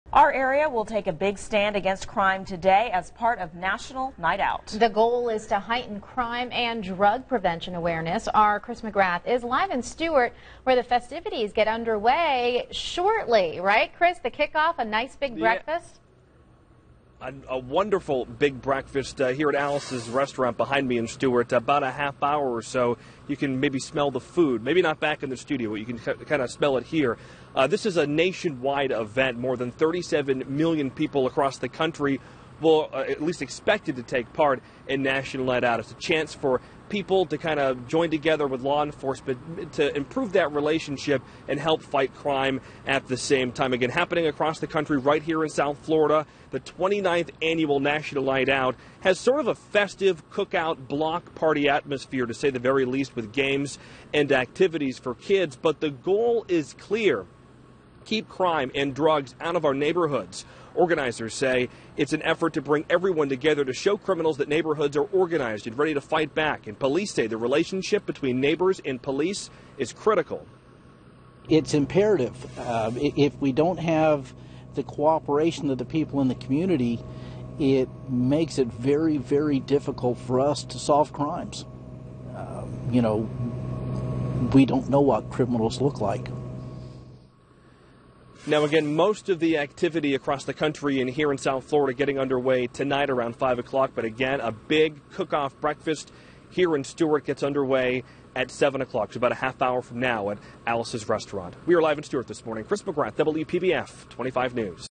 National Night Out scheduled for Tuesday
National Night Out is aimed at increasing awareness in communities of crime and drug awareness.